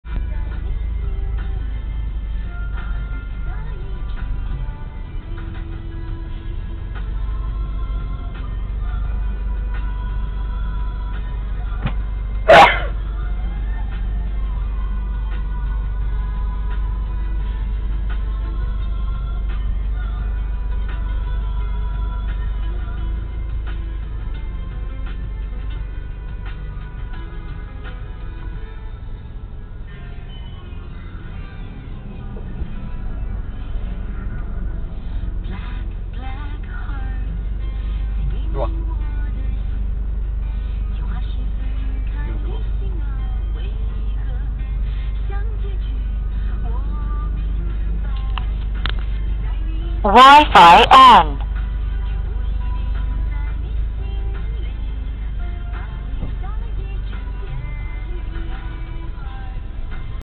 litter bug
throwing rubbish out of car windows